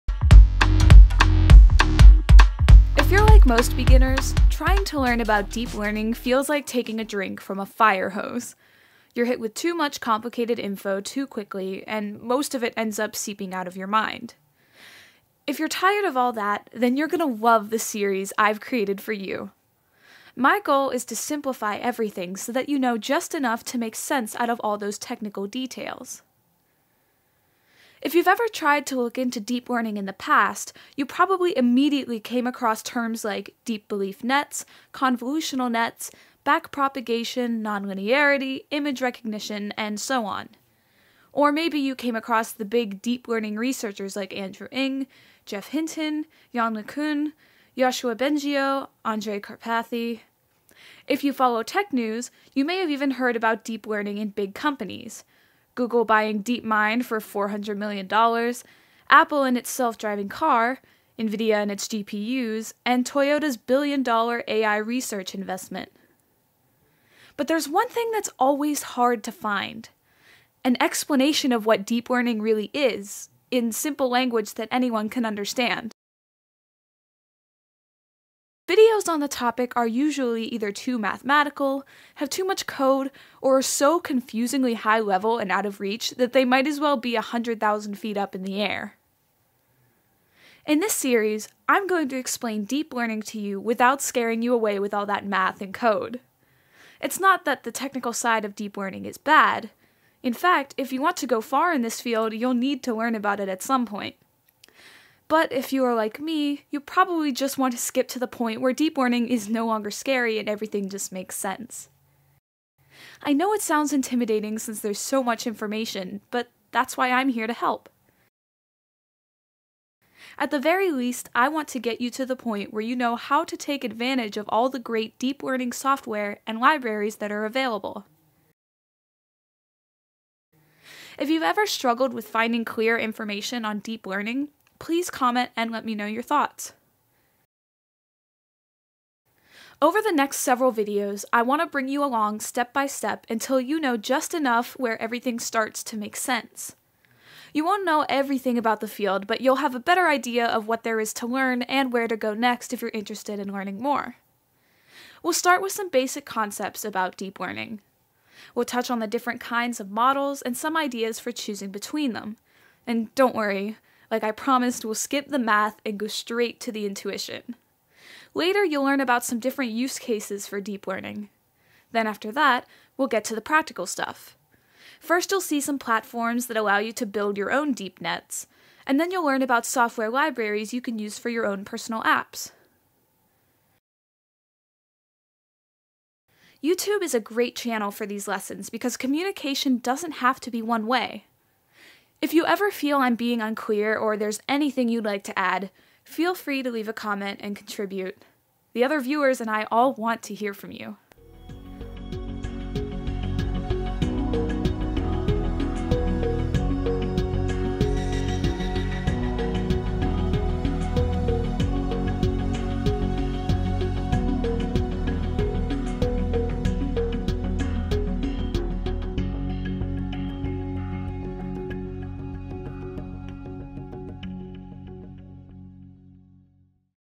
Deep Learning SIMPLIFIED: The Series Intro - Ep. 1
Are you overwhelmed by overly-technical explanations of Deep Learning? If so, this series will bring you up to speed on this fast-growing field – without any of the math or code.

Deep Learning is an important subfield of Artificial Intelligence (AI) that connects various topics like Machine Learning, Neural Networks, and Classification. The field has advanced significantly over the years due to the works of giants like Andrew Ng, Geoff Hinton, Yann LeCun, Adam Gibson, and Andrej Karpathy. Many companies have also invested heavily in Deep Learning and AI research - Google with DeepMind and its Driverless car, nVidia with CUDA and GPU computing, and recently Toyota with its new plan to allocate one billion dollars to AI research.

You've probably looked up videos on YouTube and found that most of them contain too much math for a beginner. The few videos that promise to just present concepts are usually still too high level for someone getting started. Any videos that show complicated code just make these problems worse for the viewers.

There’s nothing wrong with technical explanations, and to go far in this field you must understand them at some point. However, Deep Learning is a complex topic with a lot of information, so it can be difficult to know where to begin and what path to follow.

Does this resonate with you? What are your thoughts? Please comment.

The goal of this series is to give you a road map with enough detail that you’ll understand the important concepts, but not so much detail that you’ll feel overwhelmed. The hope is to further explain the concepts that you already know and bring to light the concepts that you need to know. In the end, you’ll be able to decide whether or not to invest additional time on this topic.

So while the math and the code are important, you will see neither in this series. The focus is on the intuition behind Deep Learning – what it is, how to use it, who’s behind it, and why it’s important. You'll first get an overview of Deep Learning and a brief introduction of how to choose between different models. Then we'll see some use cases. After that, we’ll discuss various Deep Learning tools including important software libraries and platforms where you can build your own Deep Nets.

Credits:
Nickey Pickorita (YouTube art)  -